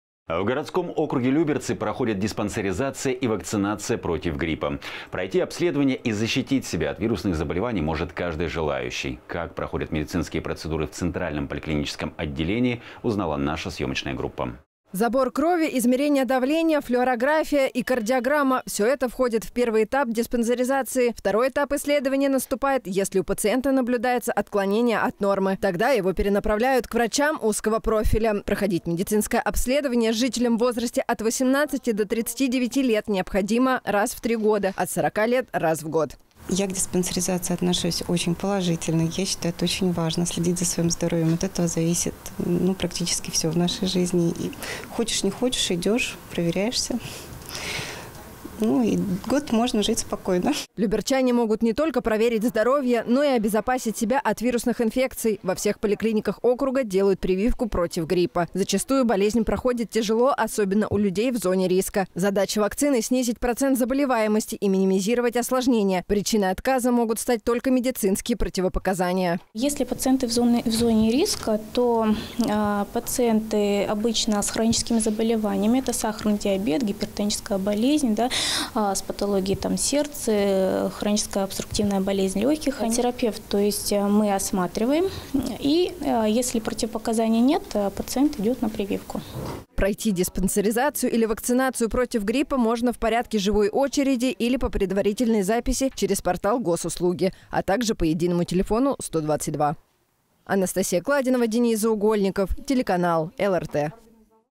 Будьте здоровы!
В городском округе Люберцы проходит диспансеризация и вакцинация против гриппа. Пройти обследование и защитить себя от вирусных заболеваний может каждый желающий. Как проходят медицинские процедуры в центральном поликлиническом отделении, узнала наша съемочная группа.

Забор крови, измерение давления, флюорография  и кардиограмма – все это входит в первый этап диспансеризации. Второй этап исследования наступает,  если у пациента наблюдается отклонение от нормы. Тогда его перенаправляют к врачам узкого профиля.  Проходить  медицинское обследование  жителям в возрасте от 18 до 39 лет необходимо раз в три года, от 40 лет – раз в год.

Анна Чуйко, жительница г.о. Люберцы: « Я к диспансеризации отношусь очень положительно. Я считаю, это очень важно – следить за своим здоровьем. От этого зависит практически все в нашей жизни. Хочешь, не хочешь, идешь, проверяешься. Ну и год можешь жить спокойно».

Люберчане могут не только проверить здоровье, но и обезопасить себя от вирусных инфекций.  Во всех поликлиниках округа делают прививку против гриппа. Зачастую болезнь проходит тяжело, особенно у людей в зоне риска. Задача вакцины – снизить процент заболеваемости и минимизировать осложнения. Причиной отказа могут стать только медицинские противопоказания.

Фарзона Ахмедова, врач-терапевт центральной поликлинической Люберецкой областной больницы: «Если пациенты в зоне риска, то это пациенты обычно с хроническими заболеваниями – это сахарный диабет,  гипертоническая болезнь с патологией там сердца, хроническая обструктивная болезнь легких. Терапевт, то есть мы, осматриваем. И если противопоказаний нет, то пациент идет на прививку».

Пройти диспансеризацию или вакцинацию  против гриппа можно в порядке живой очереди или по предварительной записи через портал Госуслуги, а также по единому телефону 122.